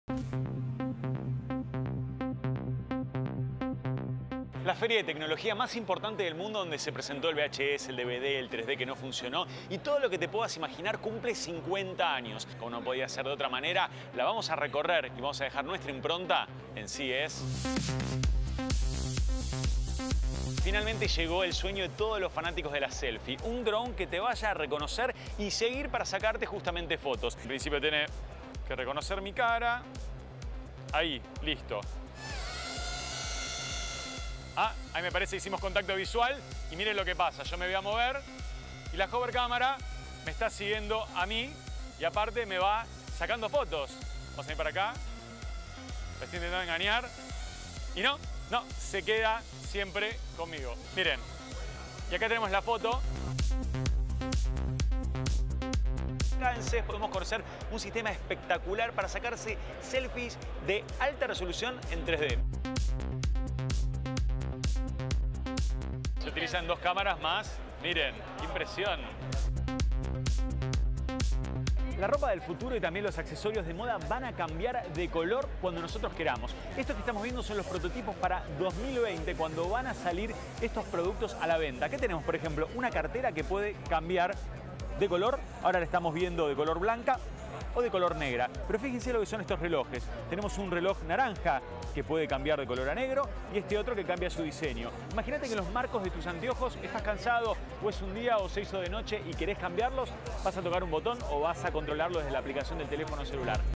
Tecnología que asombra - NOTI.20
Federico Ini visitó la feria CES 2017 de Las Vegas y nos muestra lo último en tecnología. Drone que te sigue y saca selfies y ropa que cambia de color.